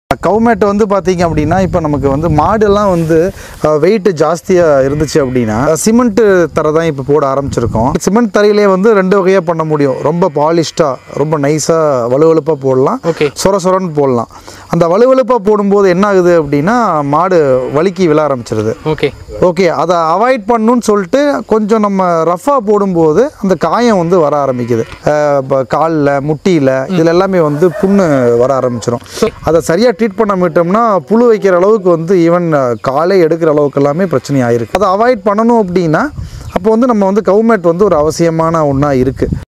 மிக குறைந்த விலையில் Cow mat
The small farm cattle WATER DRINKING BOWL is widely used in animal husbandry to provide clean water for cattle,Other than food, clean water is the most important thing for the health and well-being of your livestock. Having a quality livestock waterer will ensure that your livestock will always be hydrated and healthy with minimal effort on your part.